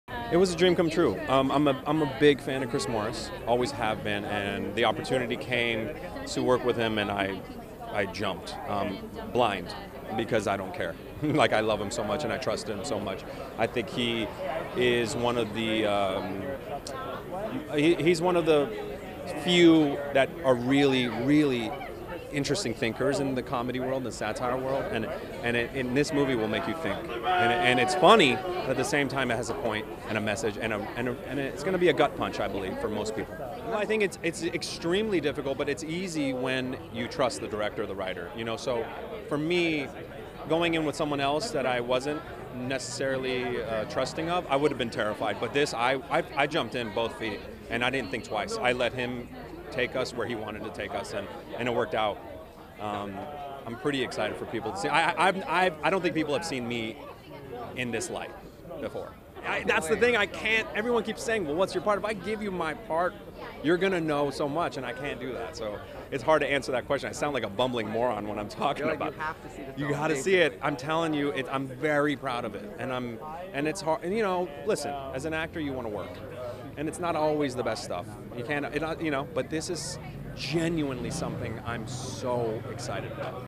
SXSW 2019: Pej Vahdat talks "The Day Shall Come"
The actor talks about his role and more on the red carpet for the SXSW premiere at the Paramount Theatre.